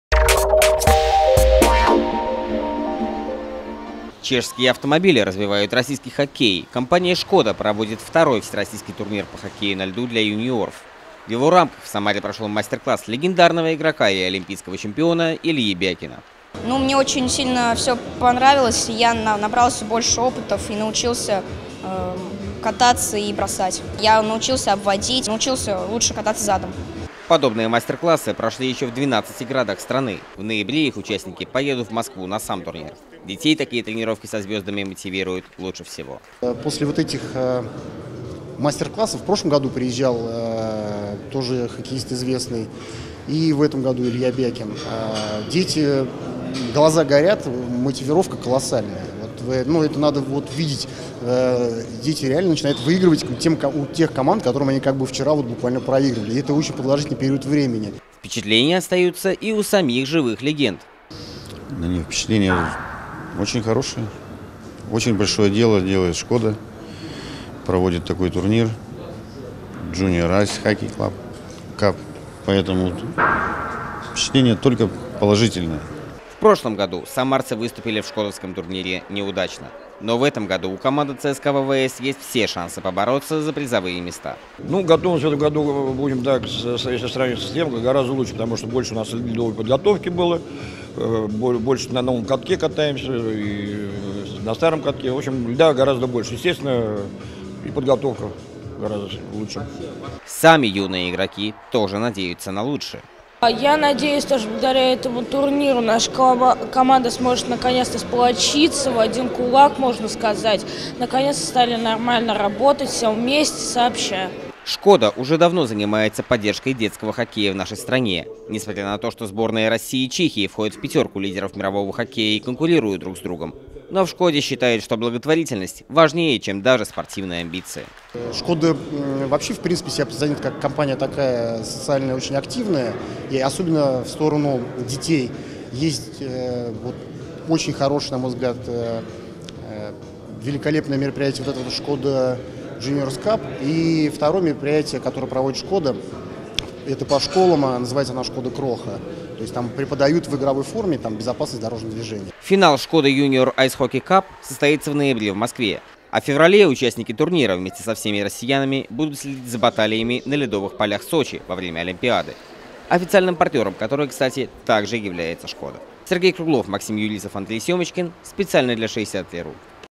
&#352;koda привезла хоккей в Самару
Чем знаменита Чехия? Пражским Градом, пенным напитком, кнедликами, богемским стеклом и трамваями «Татра». А еще автомобилями &#352;koda и своим выступлением на хоккейных ледовых полях.